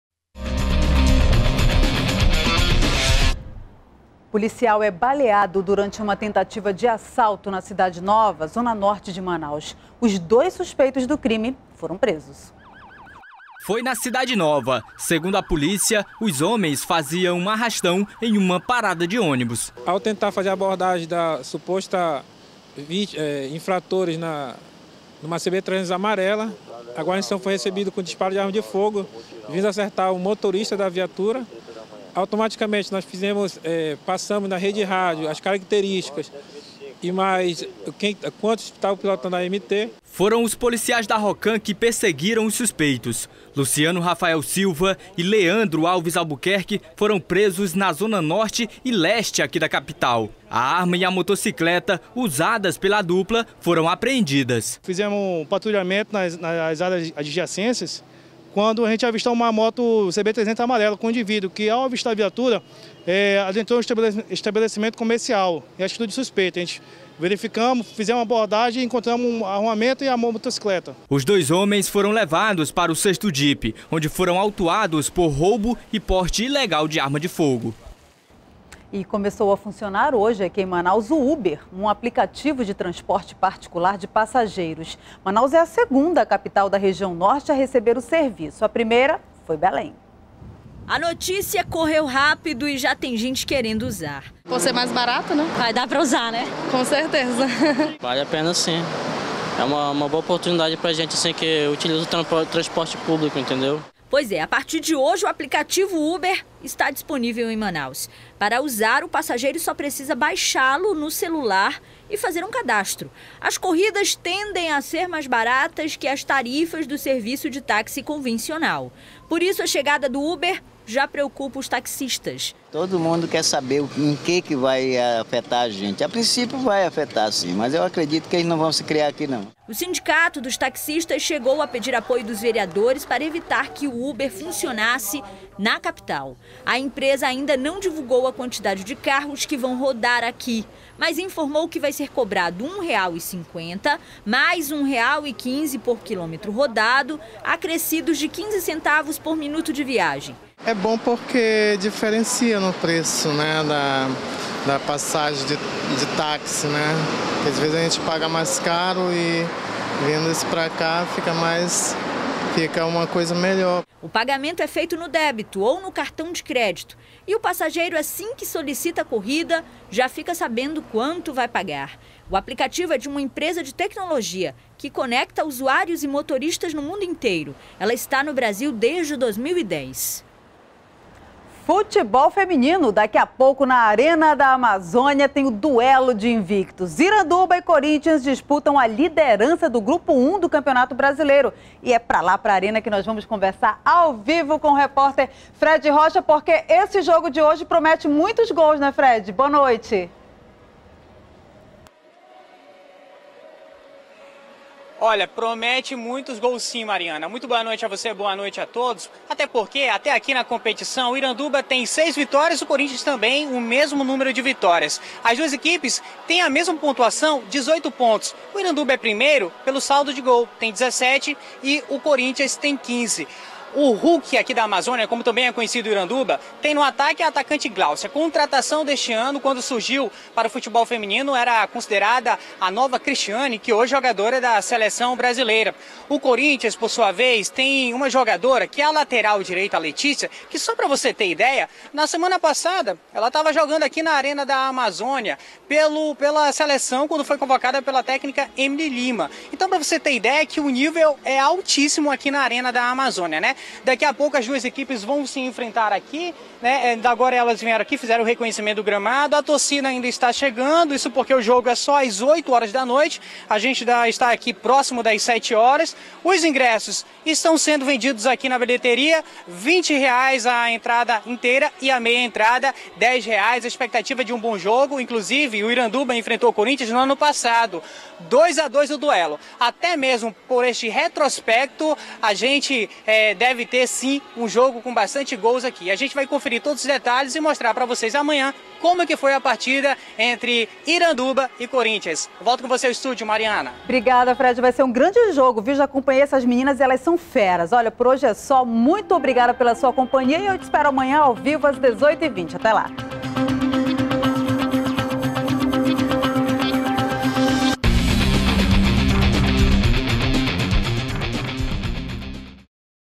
Começa a funcionar em Manaus o UBER, aplicativo de transporte particular de passageiros
03º BLOCO JORNAL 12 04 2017 - Começa a funcionar em Manaus o UBER, aplicativo de transporte particular de passageiros